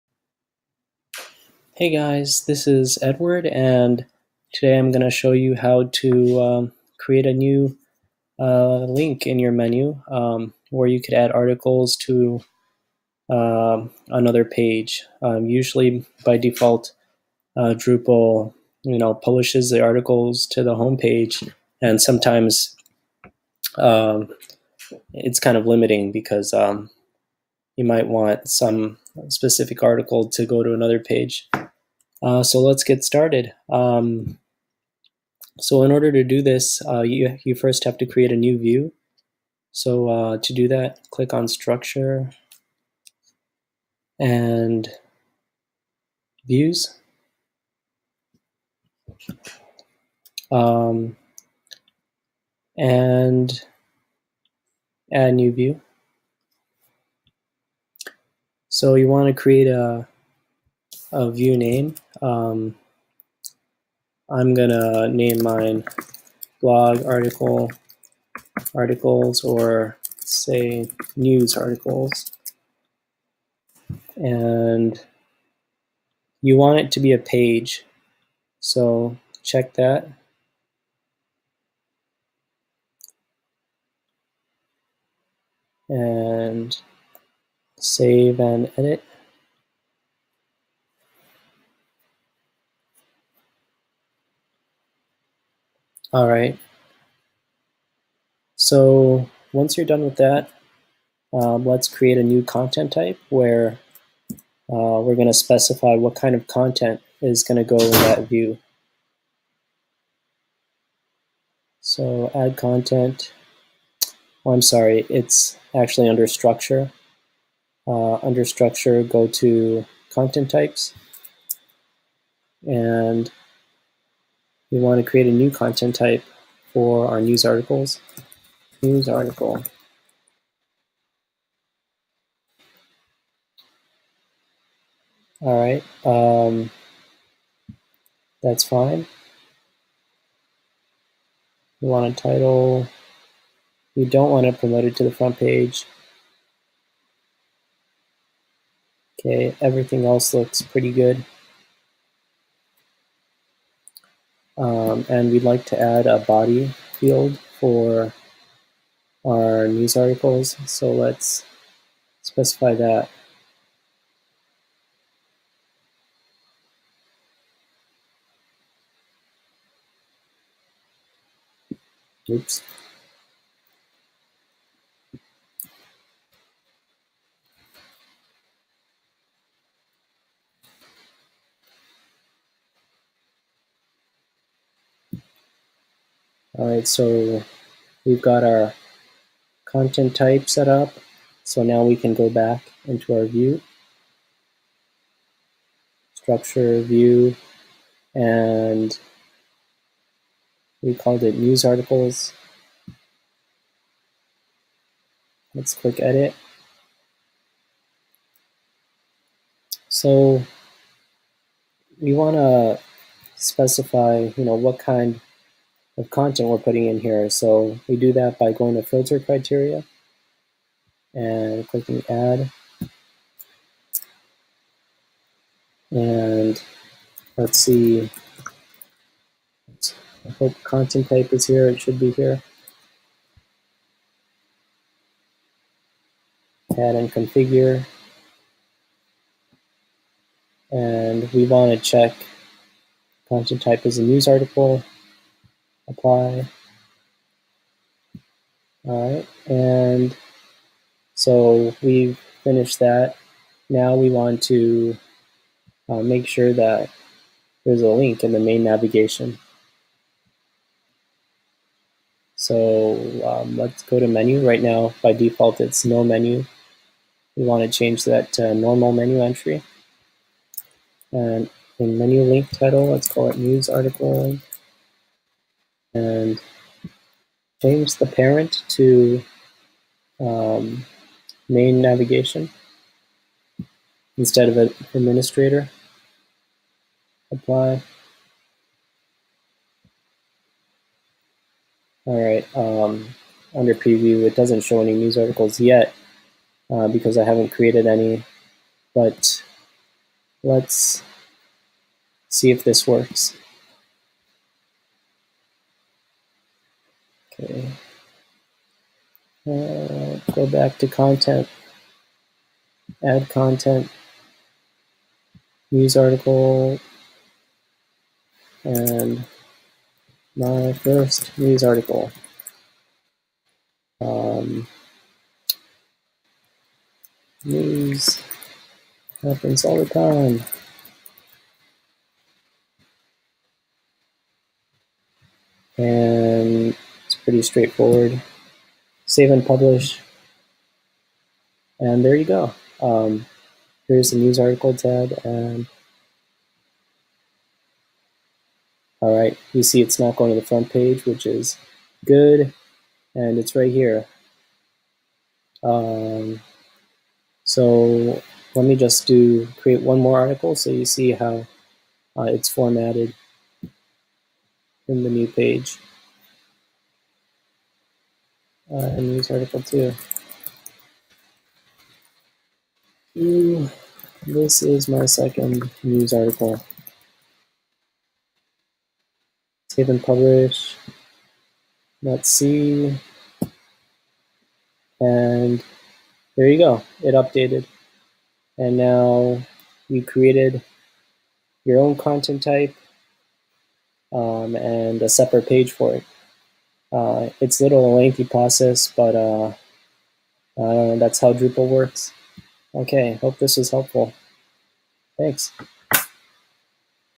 Drupal: Publish Articles Directly to New Page
How to Publish Content to a Specific Page

Here's the repost:

The answer ended up being the Views module. Download and install this module, but make sure you download and install the Chaos Tools (CTools) module first.

Once I had this enabled, I was able to achieve exactly what the OP was attemping.

For anyone seeking to do the same, here are the instructions once you have installed Views and CTools and then enabled all of their features Drupal 7 Configuration.

First you may want to create a content type for your proposed page, unless you plan on using the existing article content type. In my case, I did not want all articles to appear on the page. I only wanted certain content that I created to appear.

I wanted to create a news page, so I created a new content type called news article. This way I could designate news content to be of this special type and only appear on the news page. To create the subsequent news page, I used the Views module. Here are the instructions for what to do in Views.

1. Go into Structure
2. Click on Views near the bottom of your list
3. Ignore the list of default view items that you see on the subsequent page. Leave those alone.
4. Click 'Add new view' to create a new view with the module.
5. Use the menus to set up your page. In my case, I added a new page called News and set it up to show the news article content type. I turned off publishing to the main page and tweaked the other settings according to my preferences, and Drupal did the rest. The result was a news page that listed news articles exactly as I wanted them.